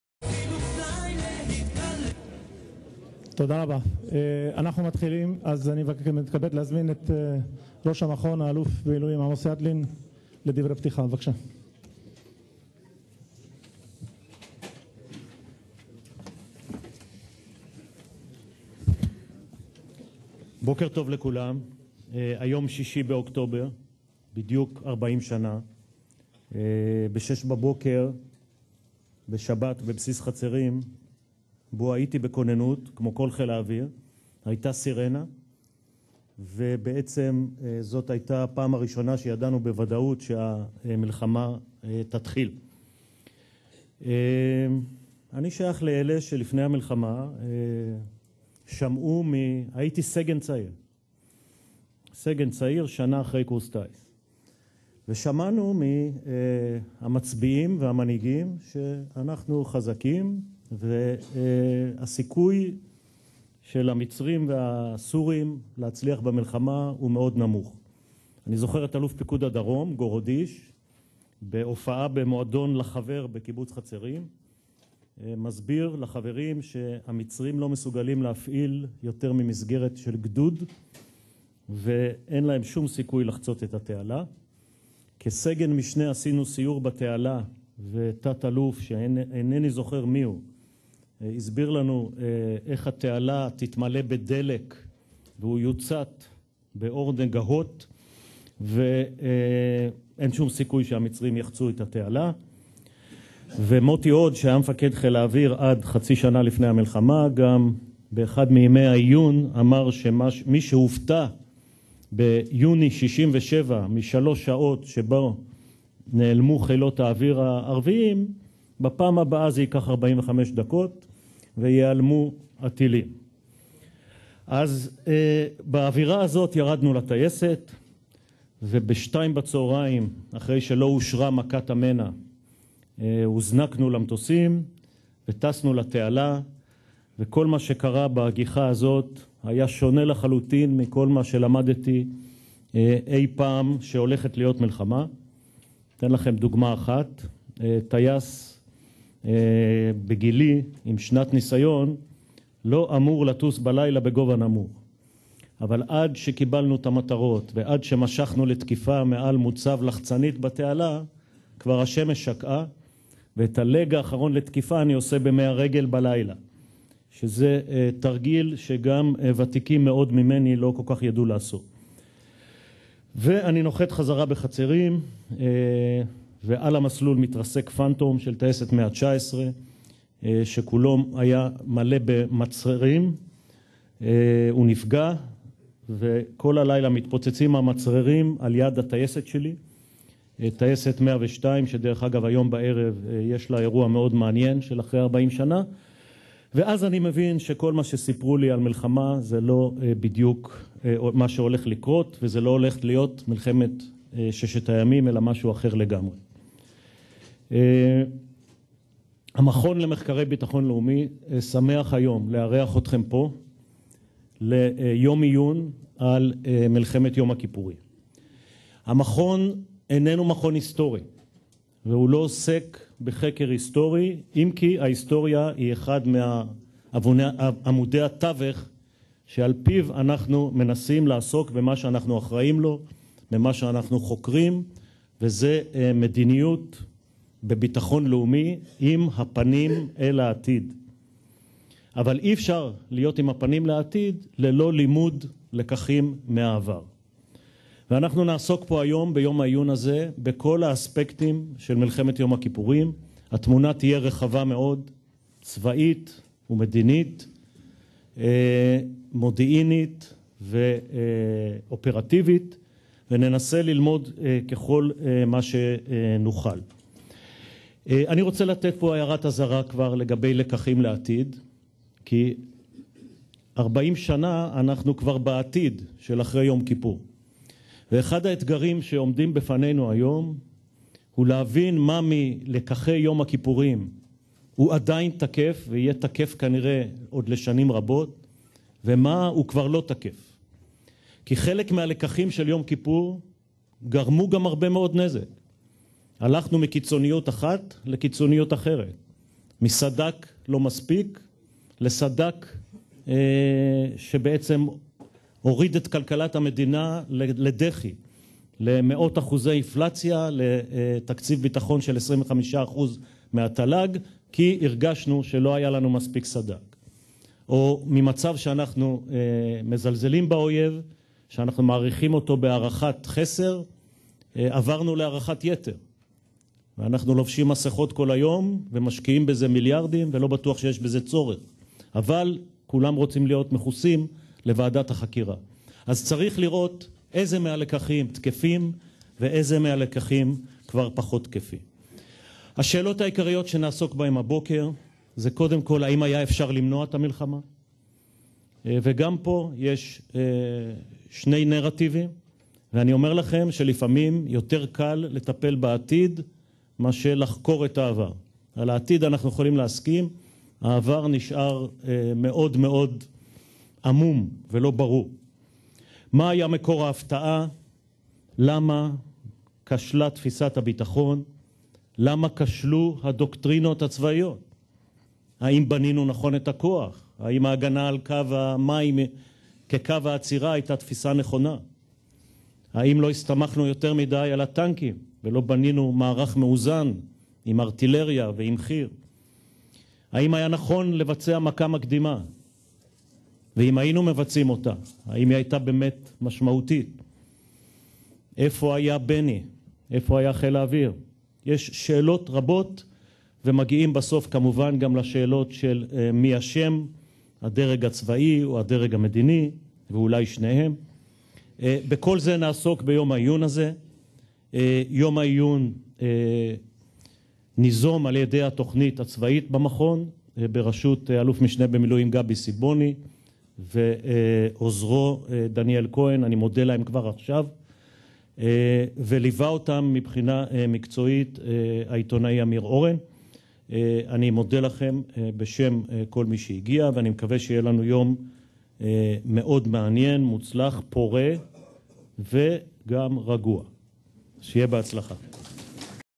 דברי פתיחה - 40 שנים למלחמת יום הכיפורים
דובר: אלוף (מיל.) עמוס ידלין, ראש המכון למחקרי ביטחון לאומי INSS
"ארבעים שנים למלחמת יום הכיפורים",
אירוע שנערך במכון למחקרי ביטחון לאומי - INSS
6.10.13